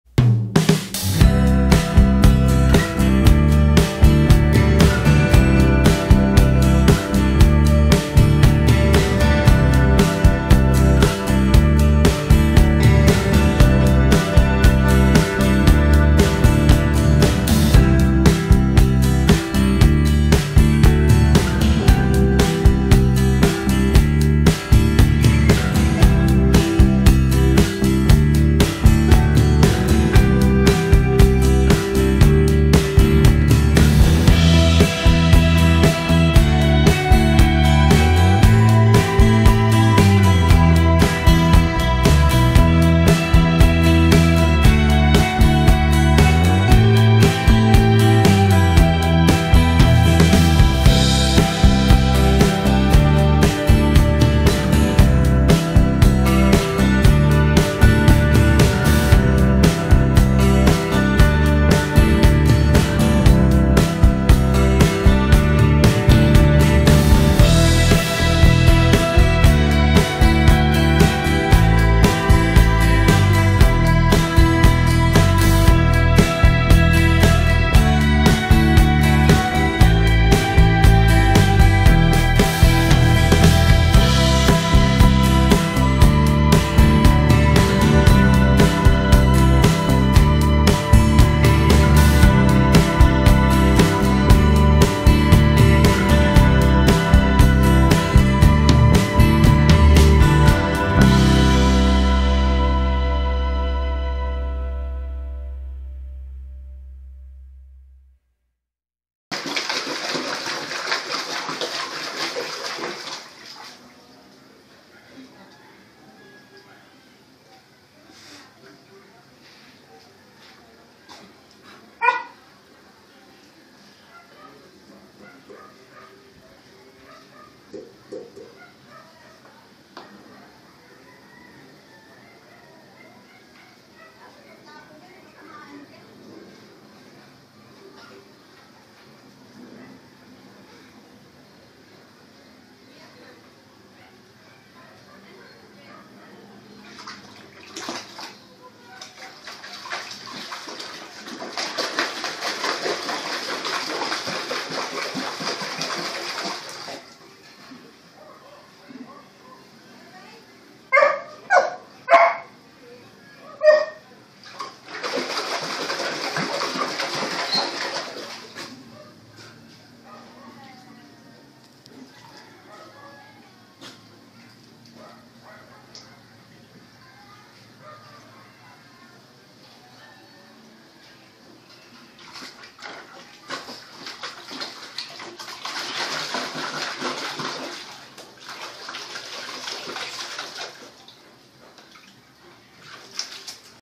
HACHICO LOVES PLAYING IN COLD WATER
sya c hachico the st.bernard mix bread hachico is 6month old dog very active he wants to take a bath in basin or (plangana) hachico likes cold water ayw nya naiinitan gusto nya palage nka aircon at mhilig makipag laro at mag lambing friendly dog sya